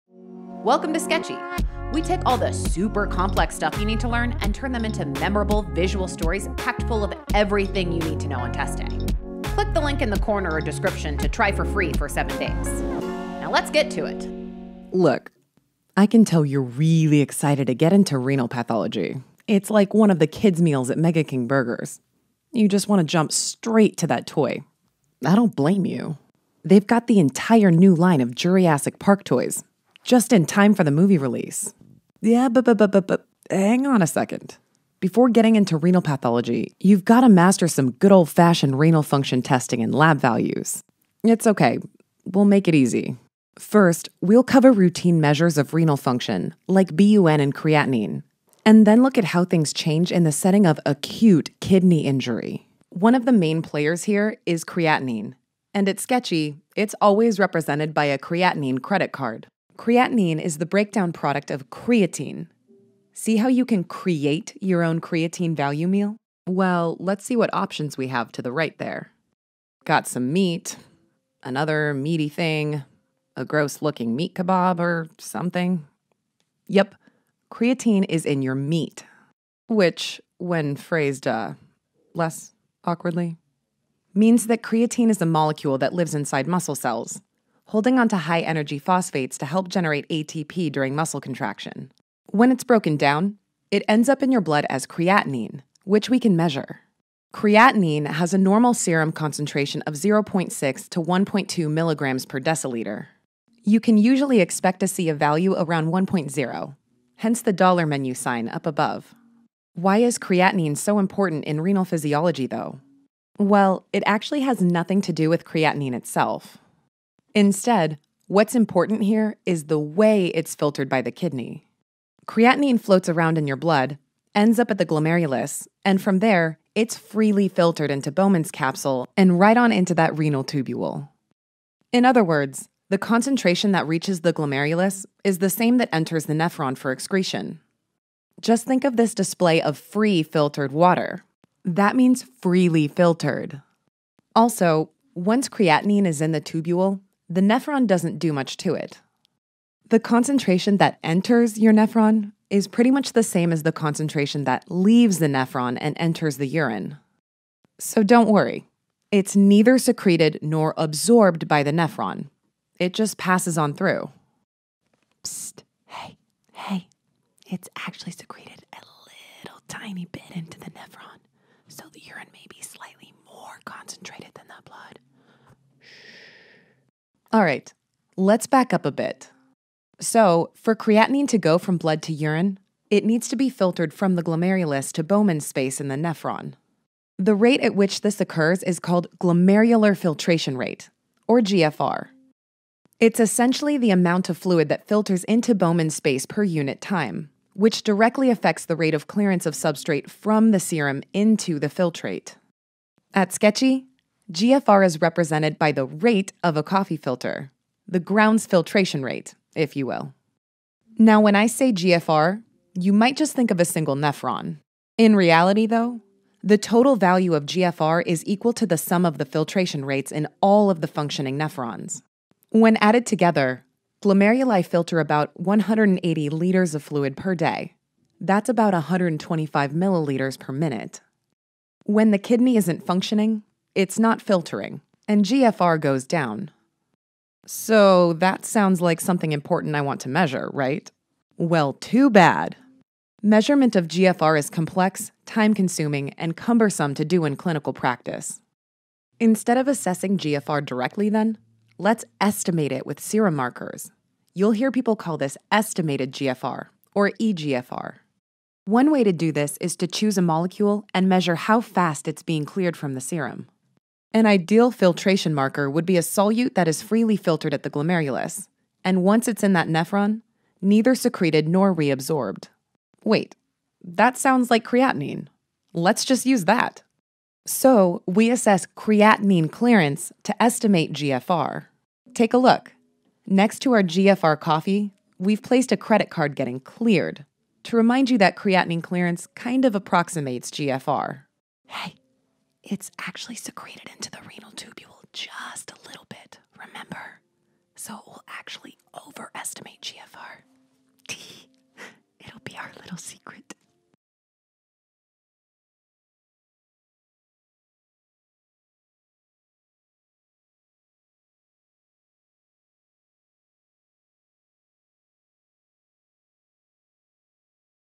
Renal Function Tests & Acute Kidney Injury (AKI) (Pathophysiology) | Sketchy Medical
This video is all about renal function testing, focusing on creatinine and blood urea nitrogen (BUN). First, we explore creatinine, which reflects kidney health by measuring filtration efficiency at the glomerulus, and how creatinine clearance, calculated using the C=U/P equation, helps approximate glomerular filtration rate (GFR). Then, we cover BUN and how it rises when GFR decreases, indicating kidney dysfunction. We discuss how the ratios of BUN/Cr and fractional excretion of sodium (FENa %) can suggest different types of kidney dysfunction, including pre-renal, intrinsic, and post-renal acute kidney injury (AKI). Understanding these tests aids in assessing kidney health and guiding clinical decisions